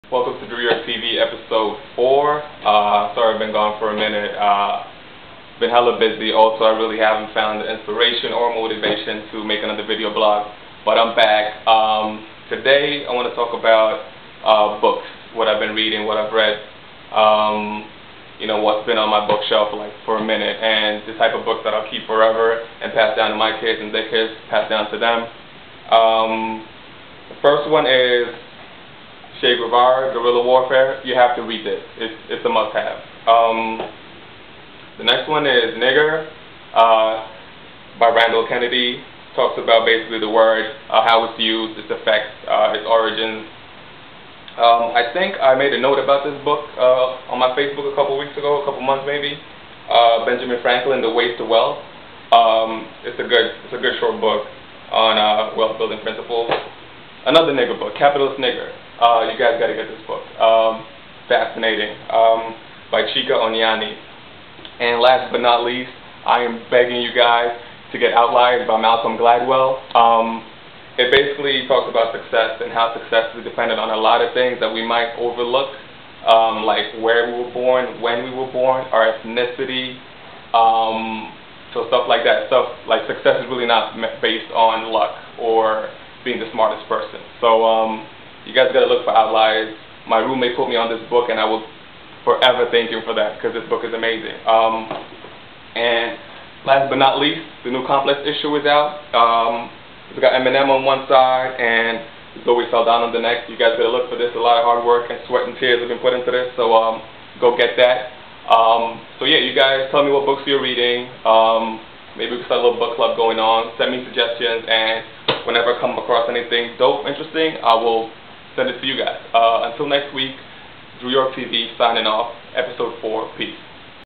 Episode 4: Books
What I've read and what I think you should read to have a better perspective on life and the things and people around us. Enjoy!